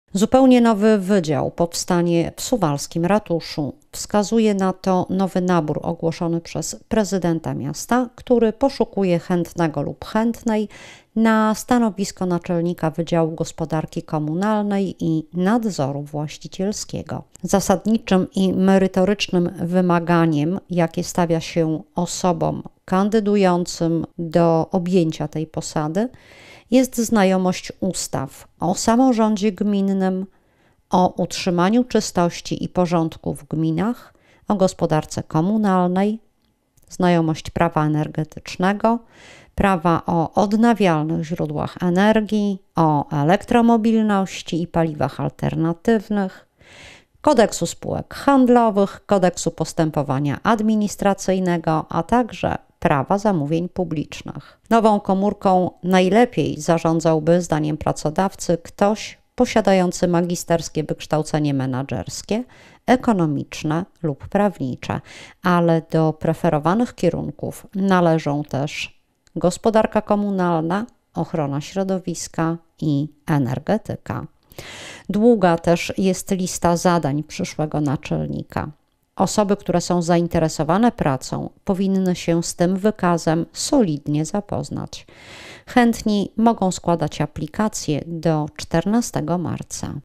Nowa posada w ratuszu. Do wzięcia. |TV SUWAŁKI
Zupełnie nowy wydział powstanie w suwalskim ratuszu. Wskazuje na to nowy nabór, ogłoszony przez prezydenta miasta, który poszukuje chętnego lub chętnej na stanowisko naczelnika wydziału gospodarki komunalnej i nadzoru właścicielskiego. Jakie są wymagania?